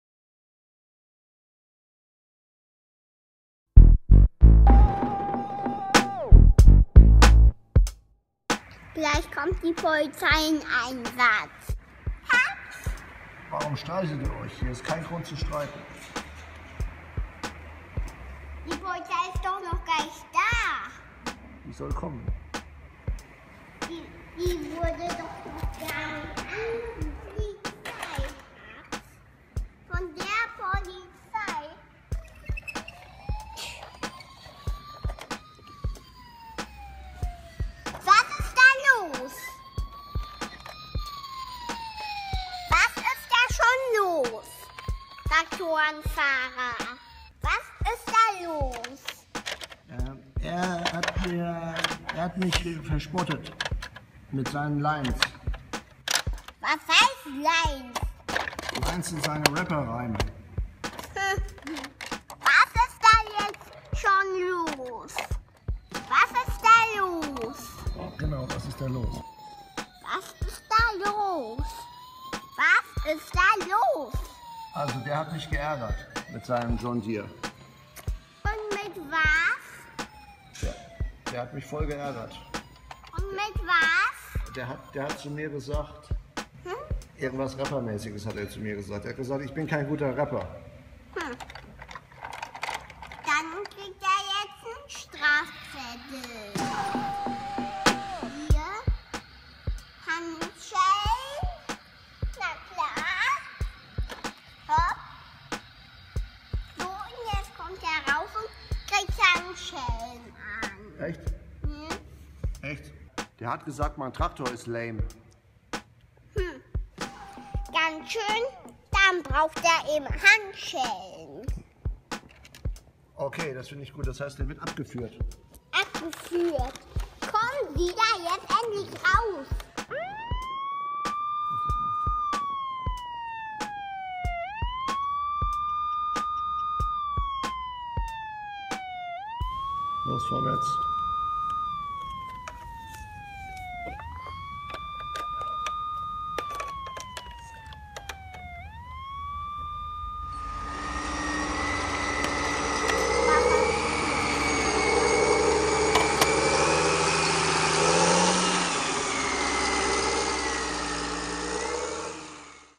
BRUDER TOYS Tractor Driver gets busted in Jack's bworld FARM
Bruder Toy videos present: BRUDER Toys TRACTOR Match CLAAS Xerion 5000 vs. JOHN DEERE 6920; driver gets busted by Bruder 02527 Jeep Wrangler Police - after stressing out each other;
Pete Ruppert Photography/Republic DRIVE;
allGlory2theLORDjesus!